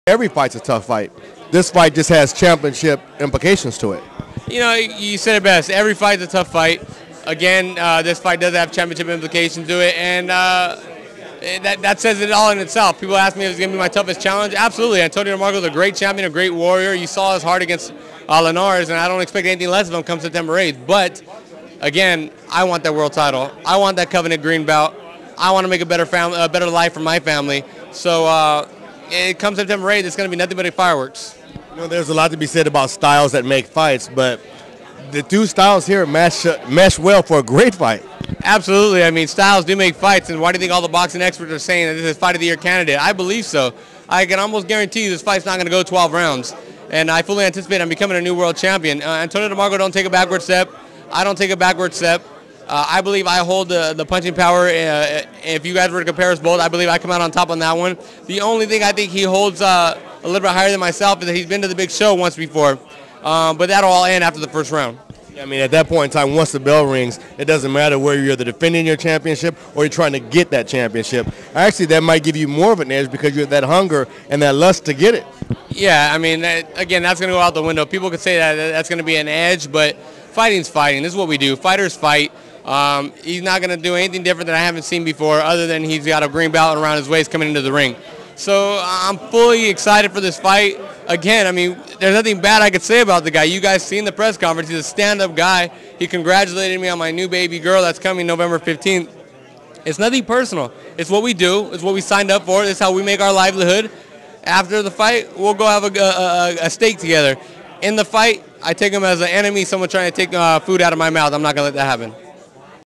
John Molina Jr. talks about fighting DeMarco for WBC Belt
John Molina Jr., will take on WBC lightweight champion Antonio DeMarco live on HBO. This co-main event is sure to be a fight fan favorite. Two fighters that have great KO power. Andre "SOG" Ward and "Bad" Chad Dawson will fight in the HBO main event in Oakland, CA. at the Oracle Arena.